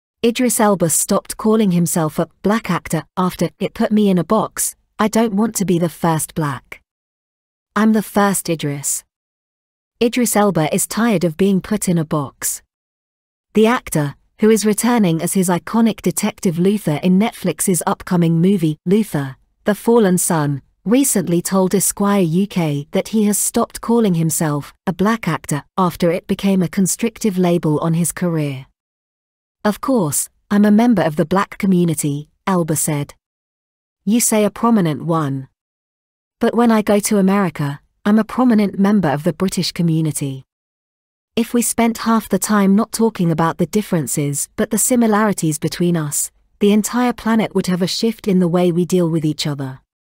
Idris Elba Opens Up About Hollywood Racism and Why He “Stopped Describing Myself as a Black Actor”
Idris Elba Stopped Calling Himself a ‘Black Actor’ After ‘It Put Me in a Box’: ‘I Don’t Want to Be the First Black. I’m the First Idris’

Idris Elba is tired of being put in a box. The actor, who is returning as his iconic detective Luther in Netflix’s upcoming movie “Luther: The Fallen Son,” recently told Esquire UK that he has stopped calling himself “a Black actor” after it became a constrictive label on his career.

“Of course, I’m a member of the Black community,” Elba said. “You say a prominent one. But when I go to America, I’m a prominent member of the British community. ‘Oh, U.K.’s in the house!’ If we spent half the time not talking about the differences but the similarities between us, the entire planet would have a shift in the way we deal with each other.”